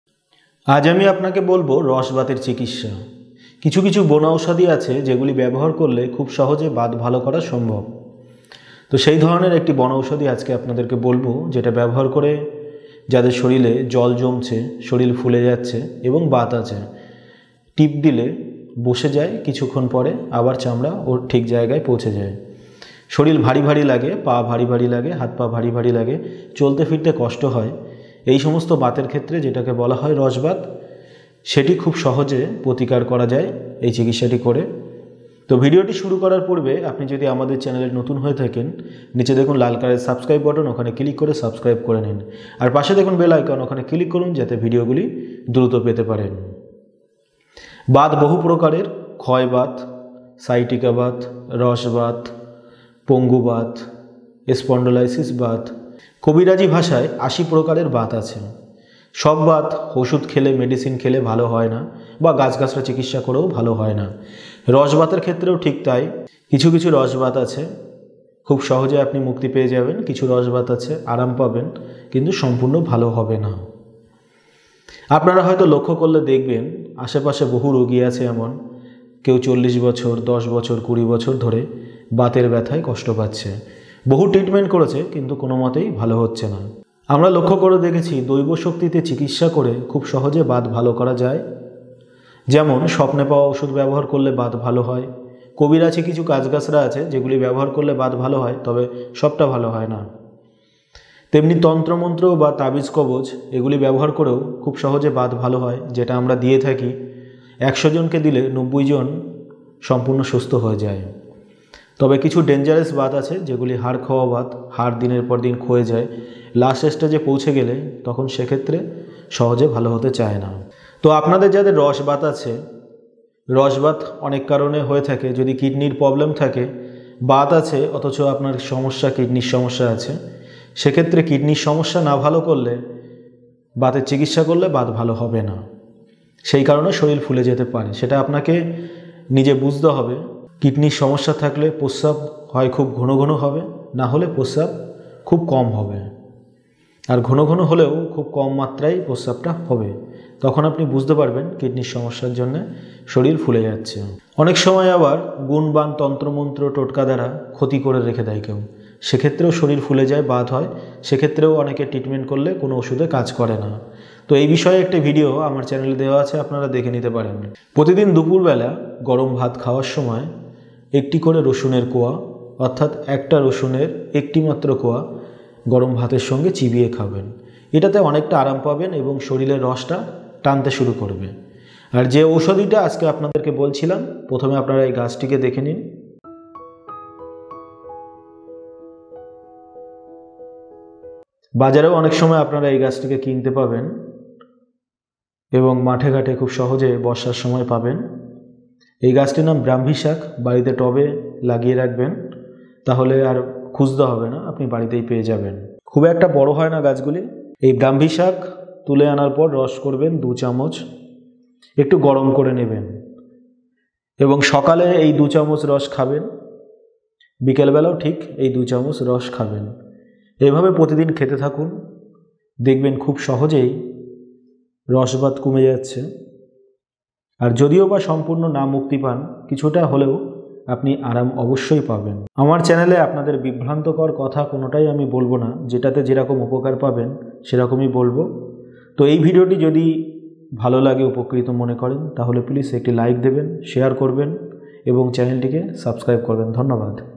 রস বাত নিরাময়ের কার্যকারী চিকিৎসা।
রস বাত নিরাময়ের কার্যকরী চিকিৎসা
এই বনৌষধি চিকিৎসা করে অল্প দিনে রস বাতের সমস্যা থেকে মুক্তি পাবেন।
রস বাত দূর করার উপায়
রস বাত ভালো করার উপায়
বাতের প্রতিকার
রস বাতের প্রতিকার